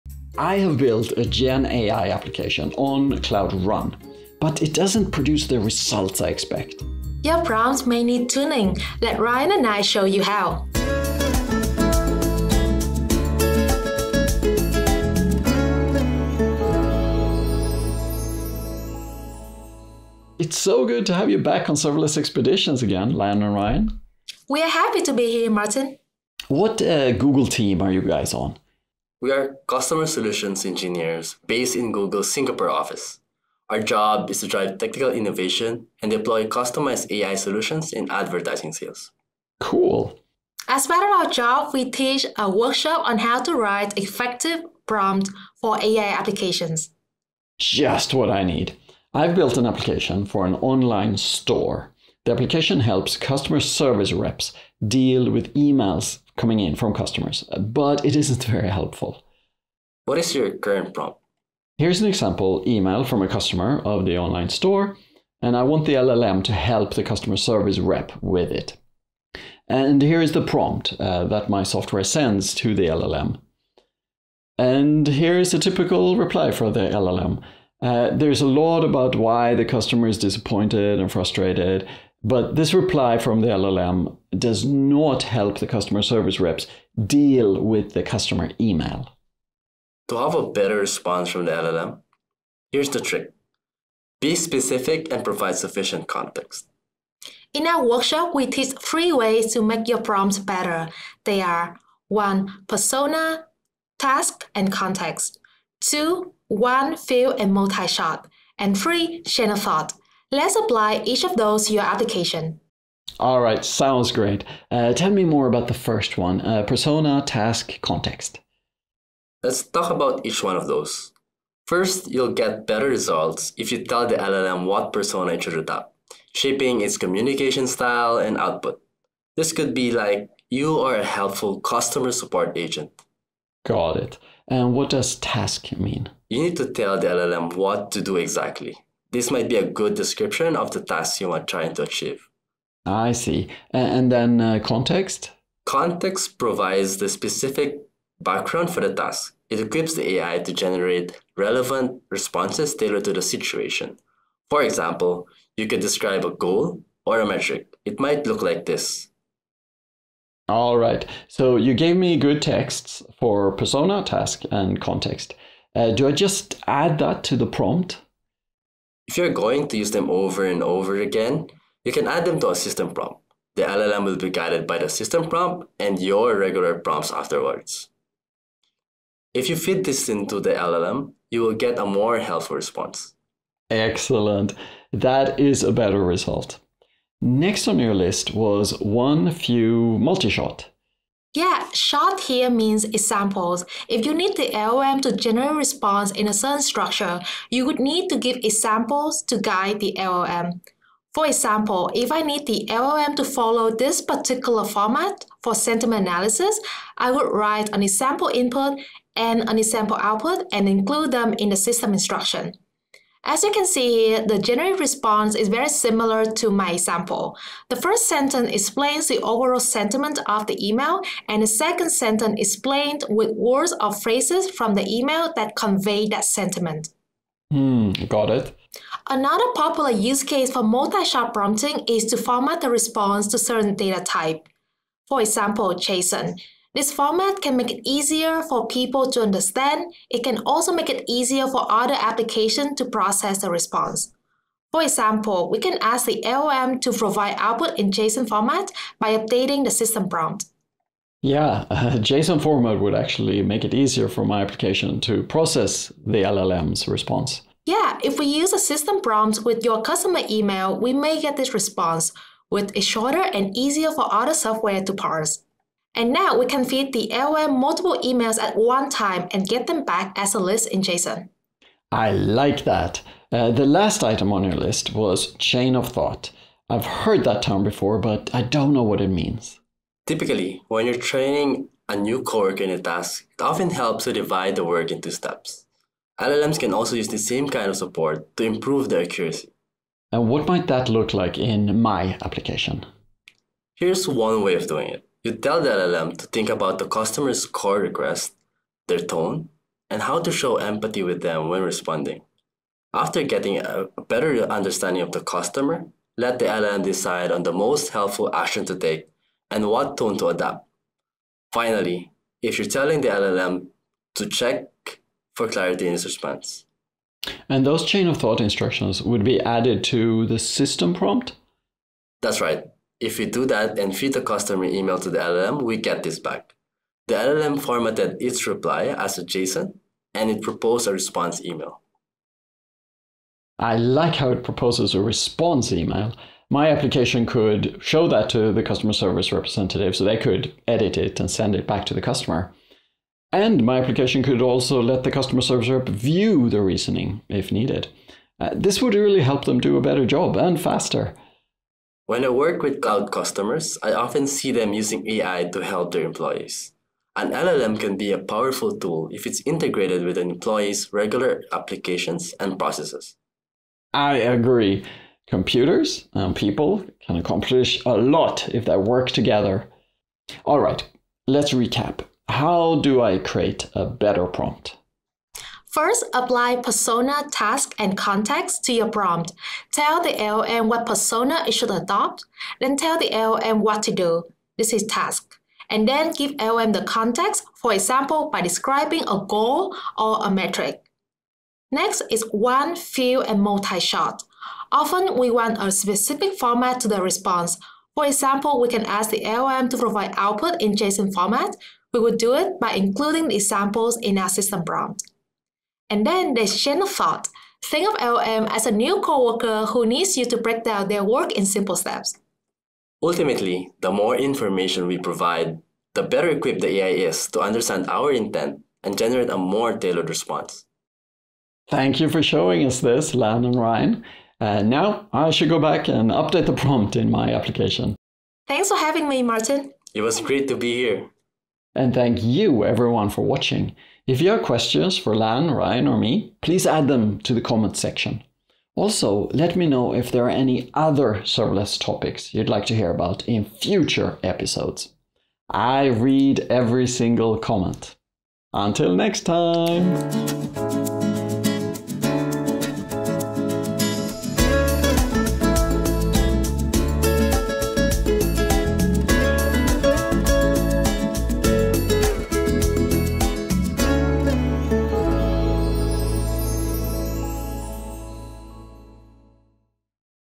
Prompt engineering for developers
Are you using chatbots and receiving high quality responses? It may be your prompt that is affecting the chatbot’s response. Take your generative AI skills to the next level by improving your prompts. Watch along and learn how to unlock more accurate and relevant responses from generative AI with chain of thought, few-shot, and multi-shot prompting techniques.

Chapters:
0:00 - Intro
0:47 - Inexperienced prompting
1:37 - Effective prompt techniques
3:06 - One-shot & few-shot prompts
4:31 - Chain of thought prompting
6:10 - Conclusion

Speaker: Martin Omander, Lan Tran, Ryan Sibbaluca
Products Mentioned: Cloud Run, Gemini